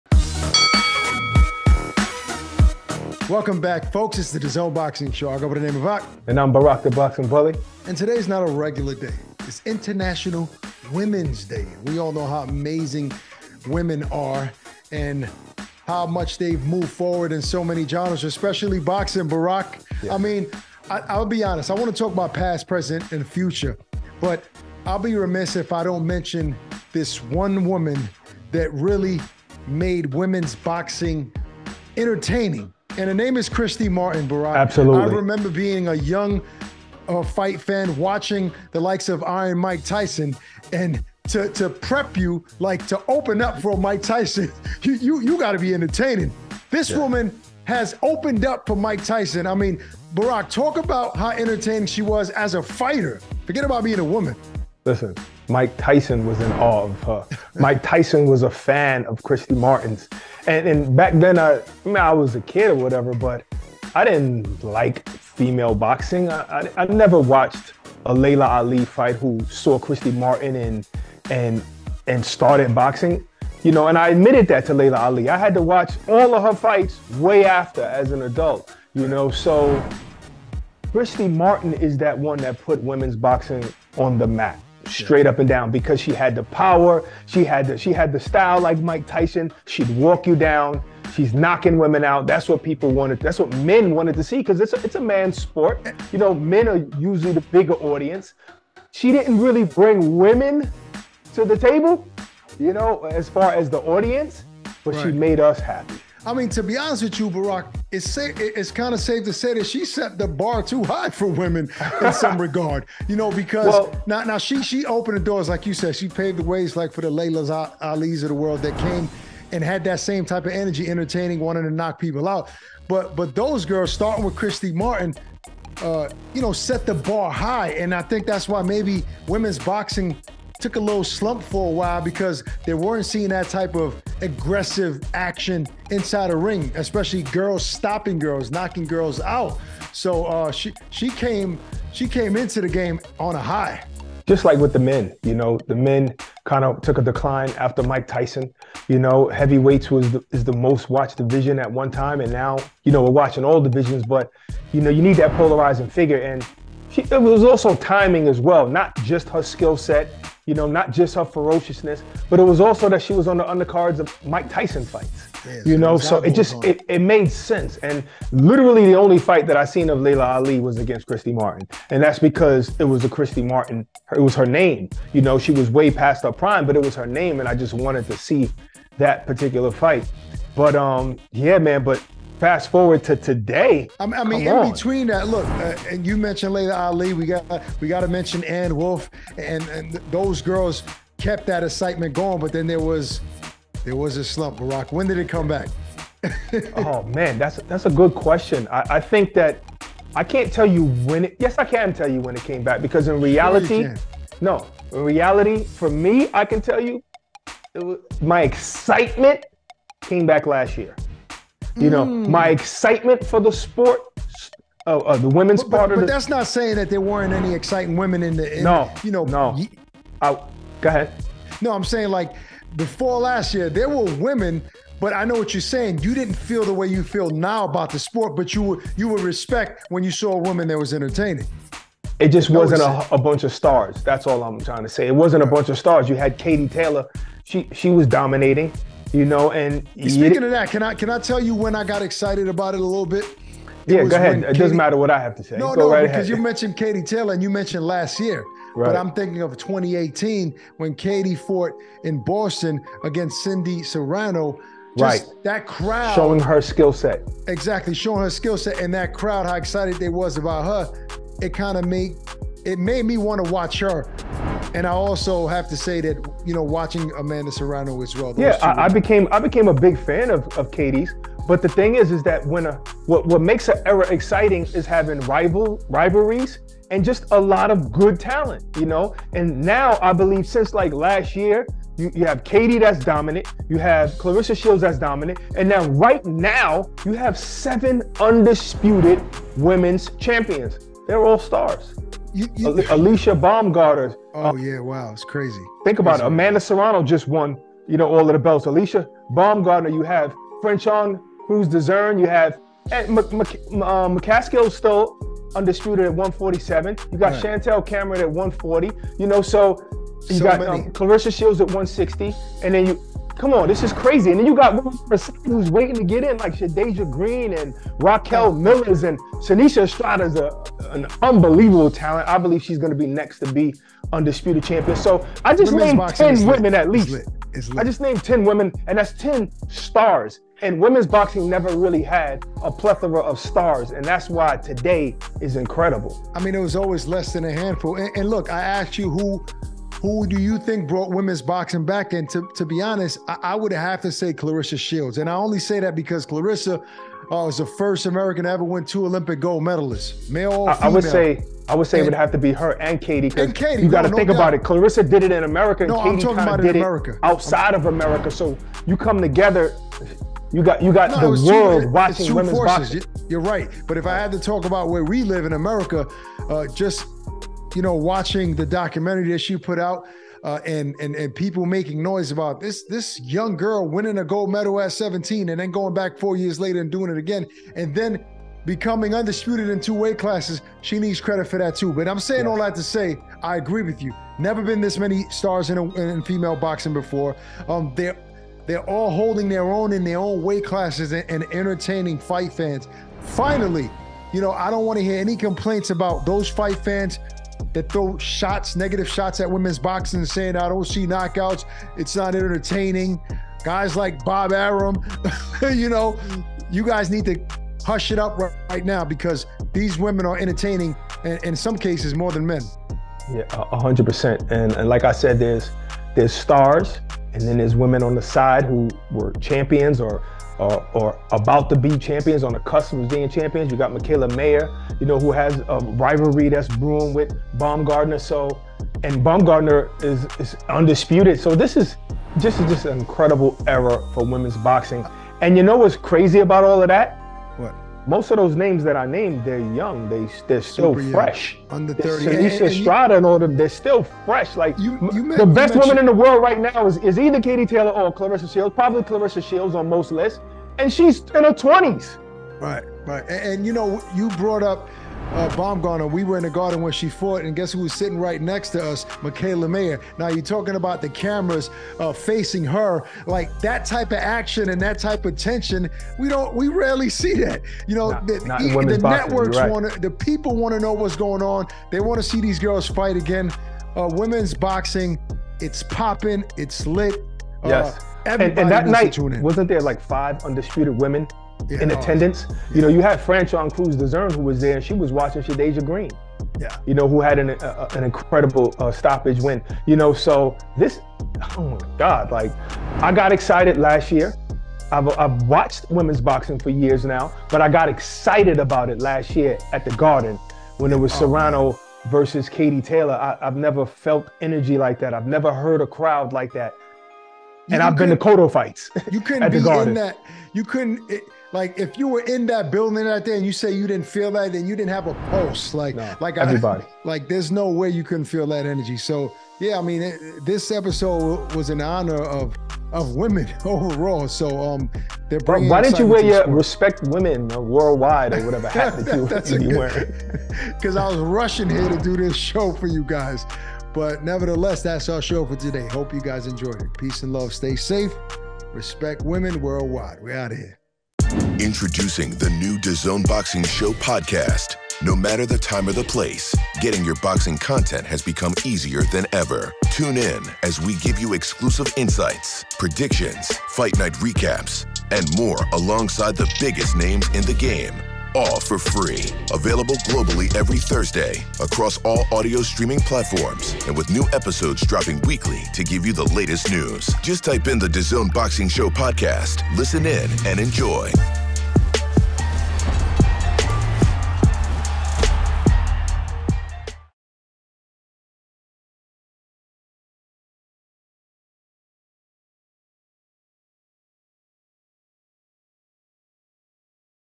Saluting The Seven Female Undisputed Champions | International Women's Day 2023 Special
On International Women's Day 2023, Ak and Barak discuss the past, present and future of women's boxing.